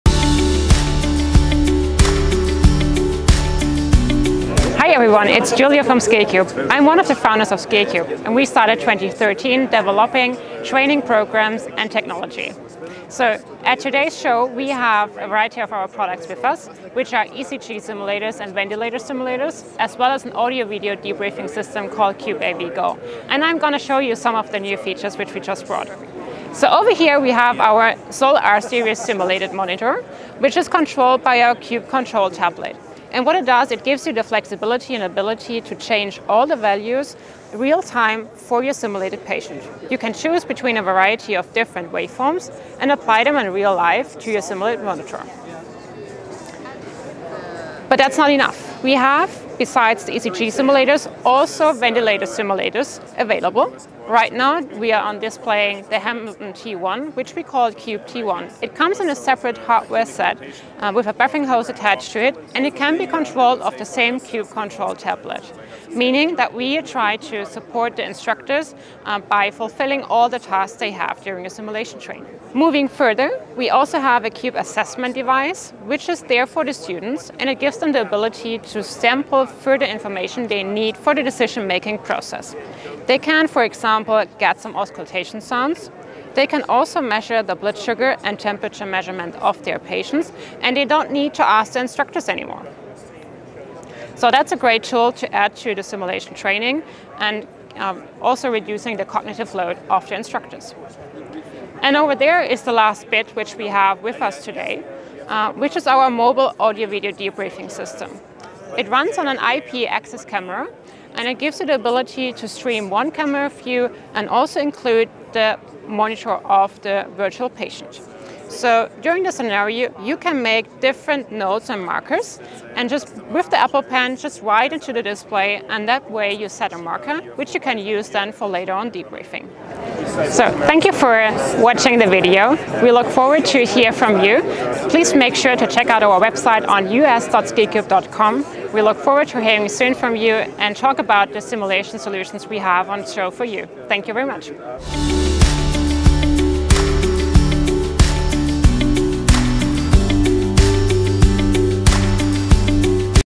SkillQube Showcases Latest Simulated Medical Tablets at IMSH 2024
was on hand at IMSH 2024 to learn the latest HealthcareSimulation Simulated Medical Device technologies from @skillqubetraining8161 .